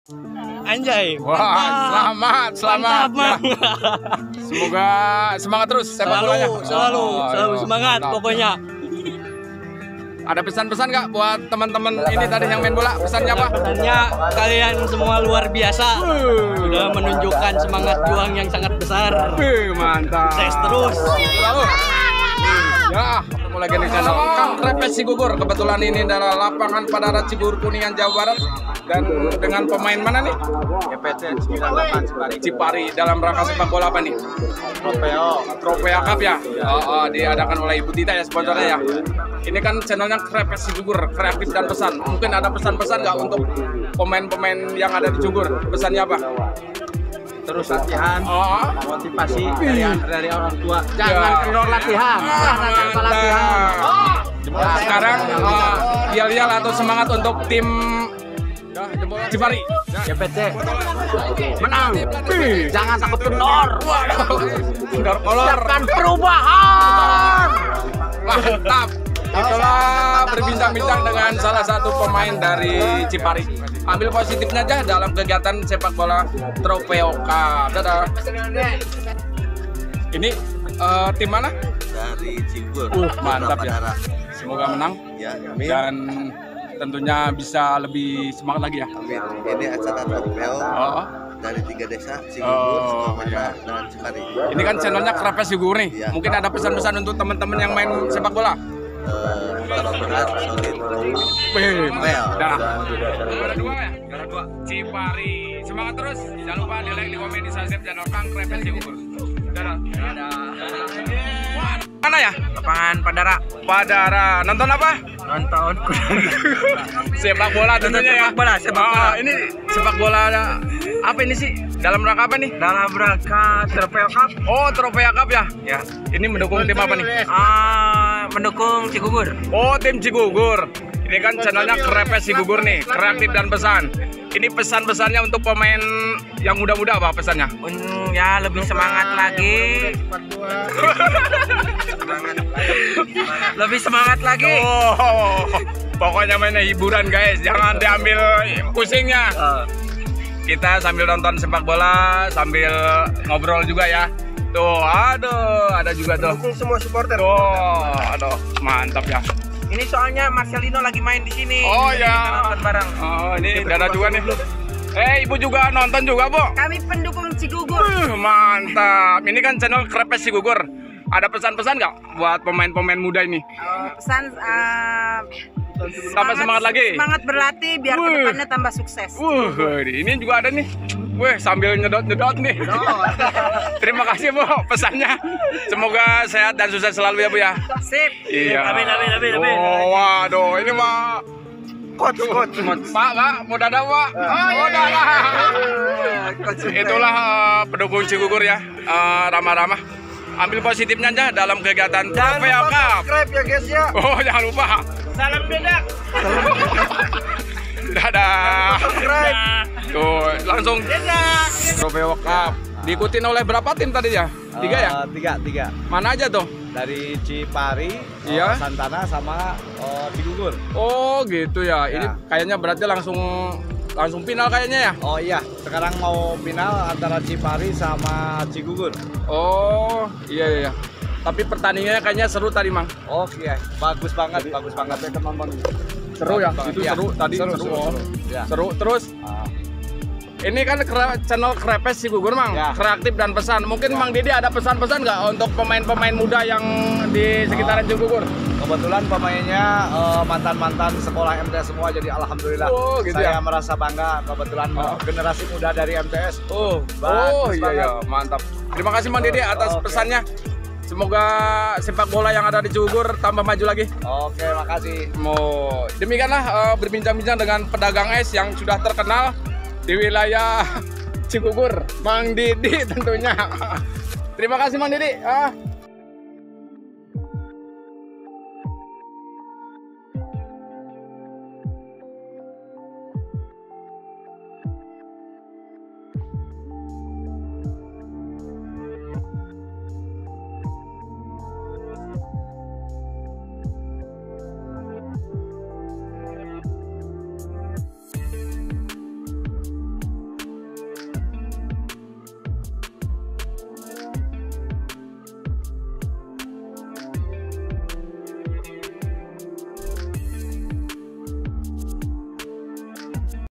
Pembagian hadiah juara Trofeo cup di lapangan Padara//Crepes cigugur bola
Pembagian hadiah sang juara dan pesan dan kesan du acara sepak bola Trofeo cup cigugur kuningan Jabar.